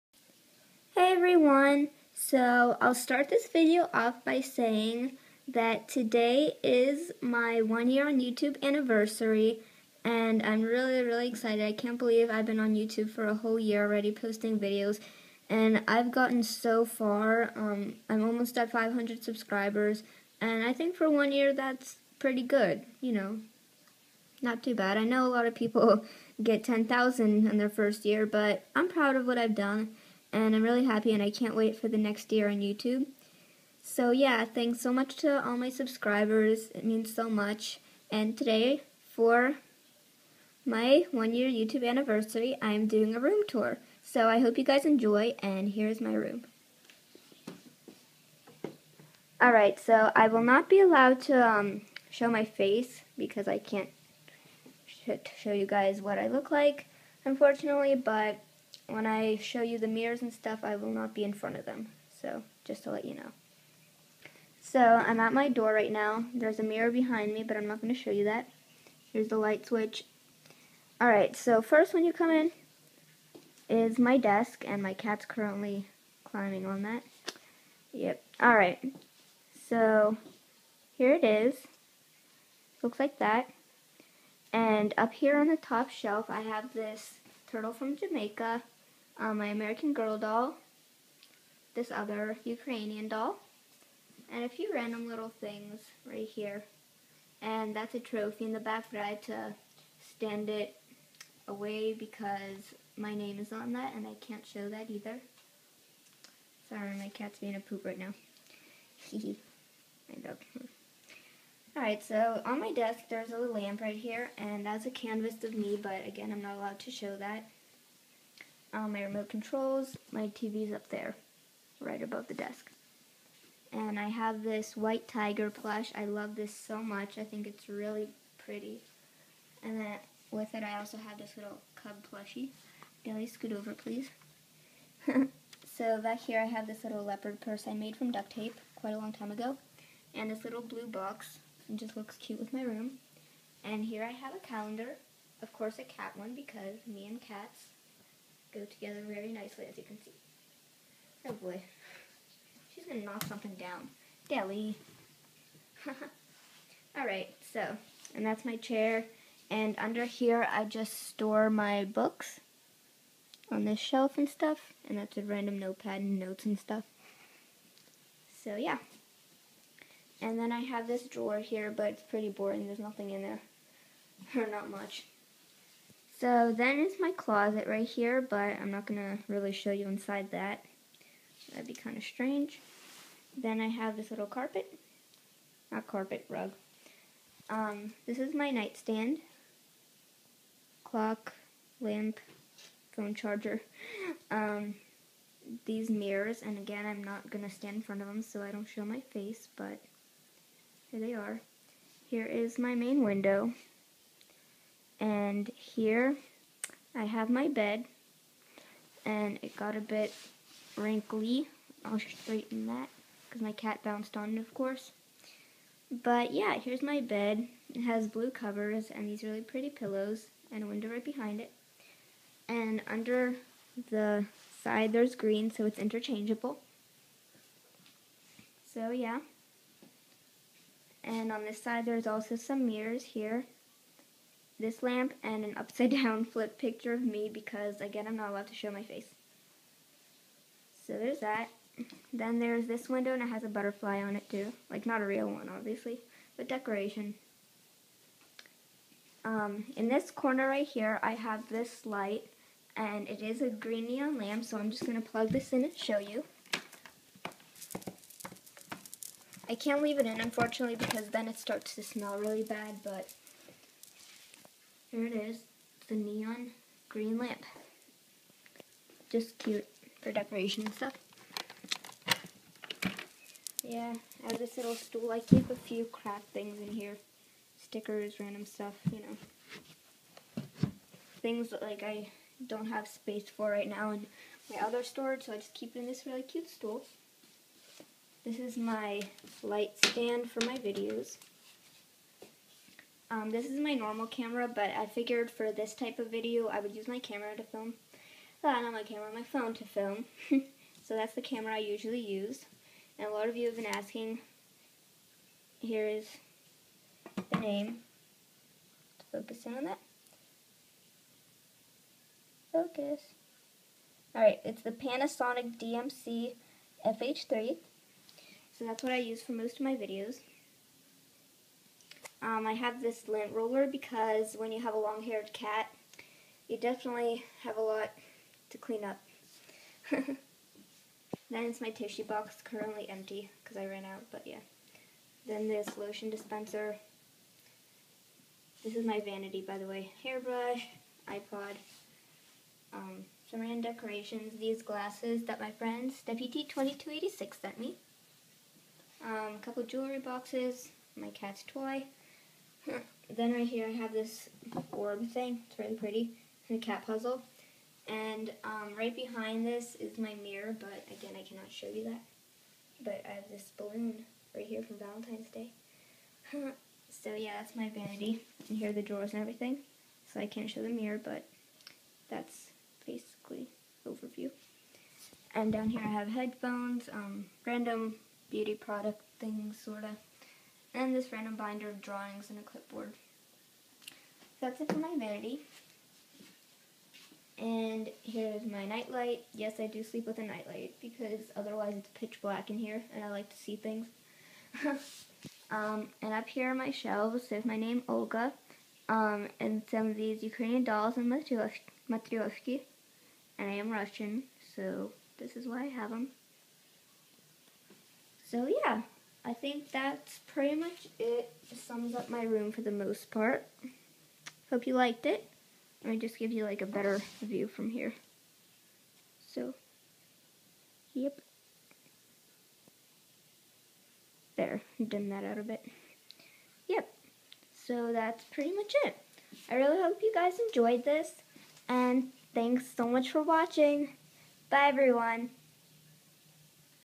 My Room Tour!!! 1 Year on YouTube Anniversary!!!! :D
I honestly can't believe I've been on youtube for a year! Thanks for all the support guys, I love every one of my subbies! :) So for my 1 year anniversary I decide to do the long awaited room tour video! This was requested a LOT haha. Please comment video ideas down below so that I can improve my channel even more this year, by posting the videos that YOU guys want to see :)